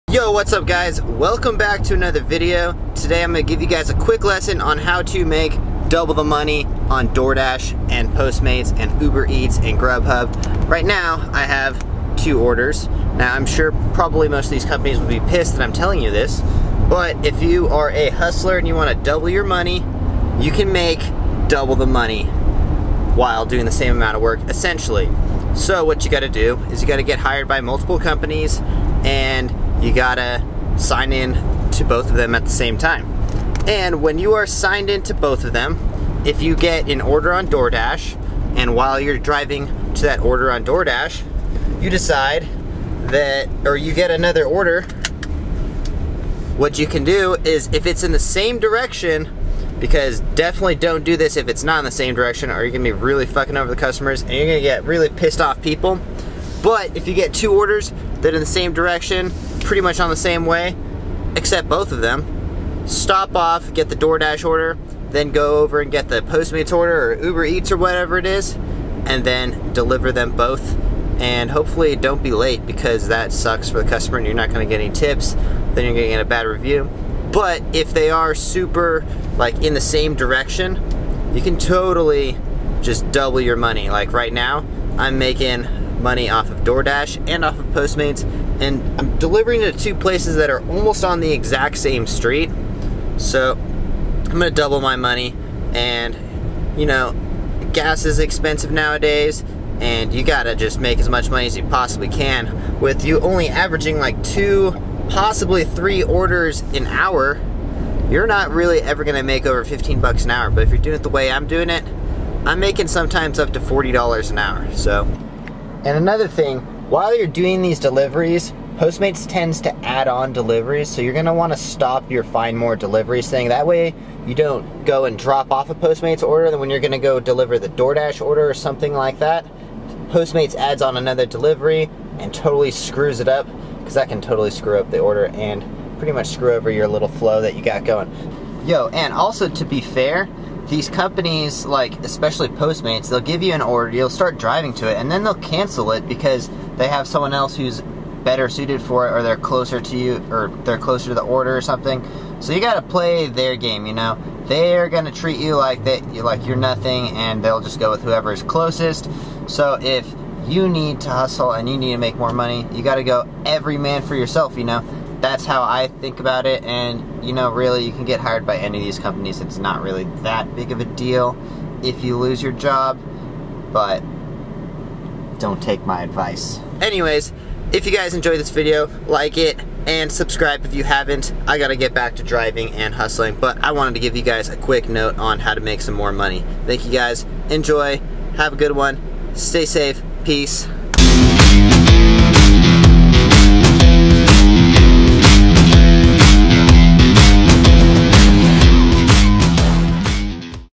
HOW TO LITERALLY DOUBLE YOUR MONEY DRIVING FOR POSTMATES, DOORDASH, GRUBHUB, UBEREATS
If you're trying to make more money driving for Doordash, Postmates, Ubereats, Grubhub, etc. this is what you need to do to double your money. Of course do it responsibly because customer service comes first.